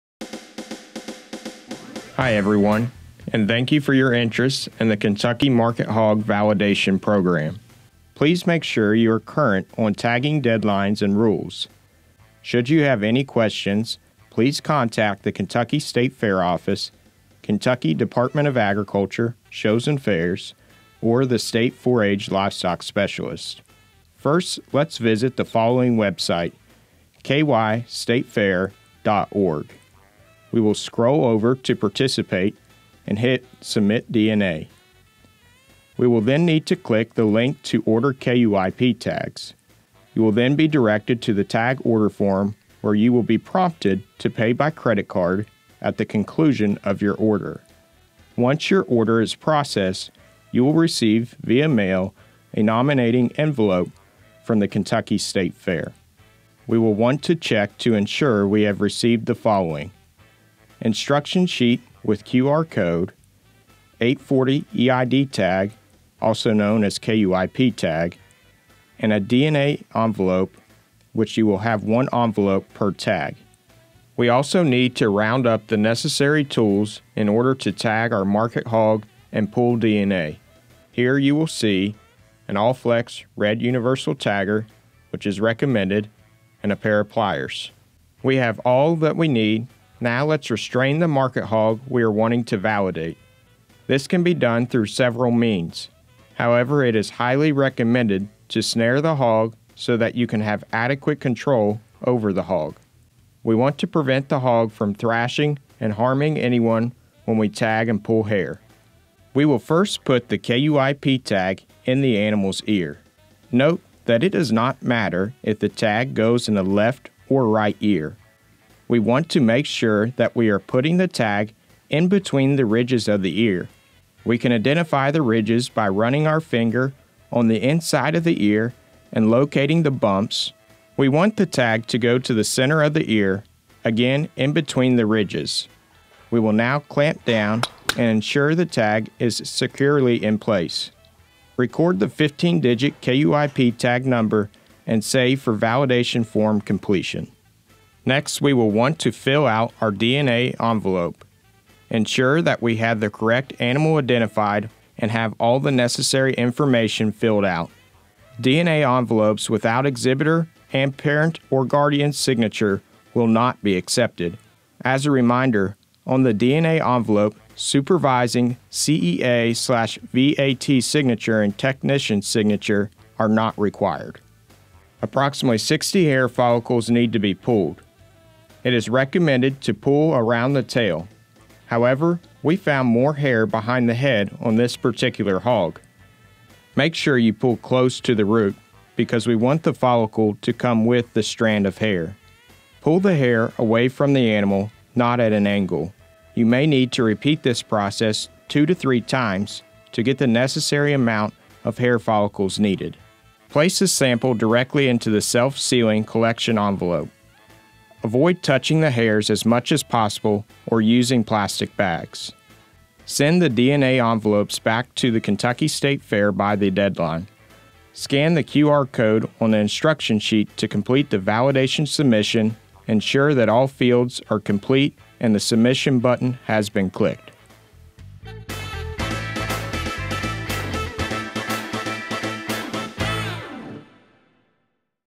Kentucky Market Hog Validation Program
Zach Bartenslager, Youth livestock program associate, shows us the tools and procedures needed to collect DNA hair samples, tag a hog, and fill out the form to enter a market hog at the Kentucky State Fair.